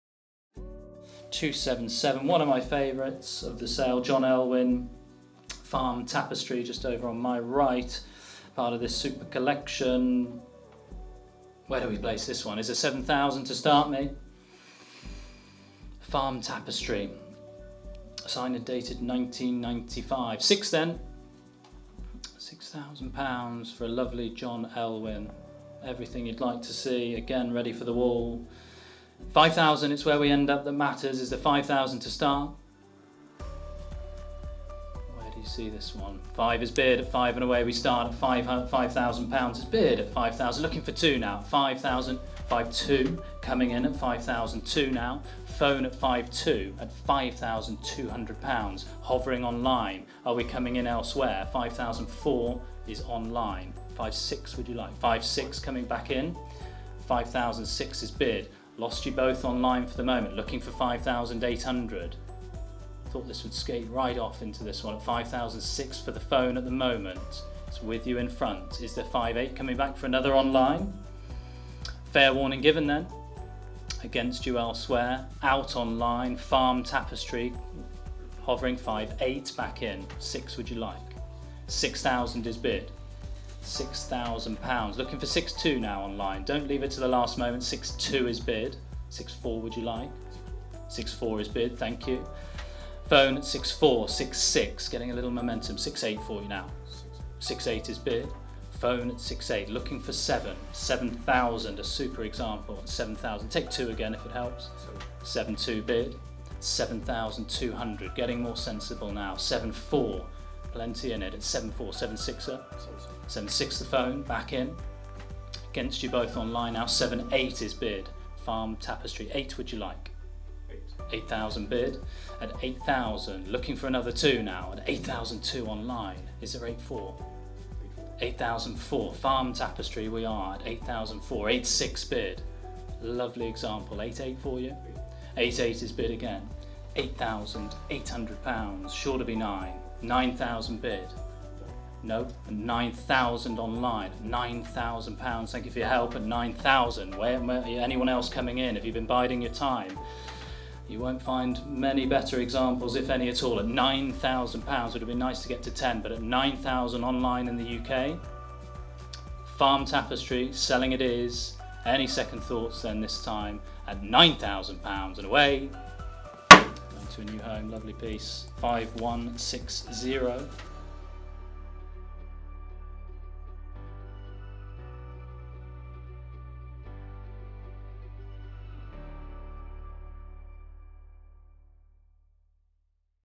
John Elwyn (Welsh Sale auction: Sept 12 2020)
Charles Hampshire oversees a fierce online and telephone battle for a simply wonderful John Elwyn oil painting